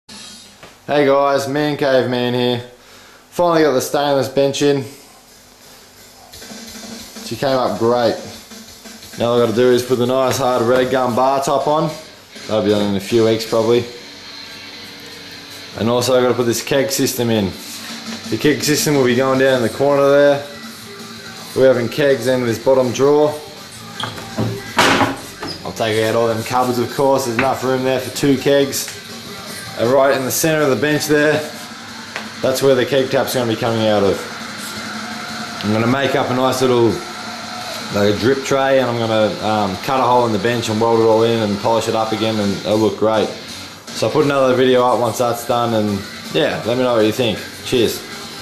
Man Cave Bar, STAINLESS STEEL
G'day guys, Just a short video on how my bars going. Showing you the finished product of my stainless steel bench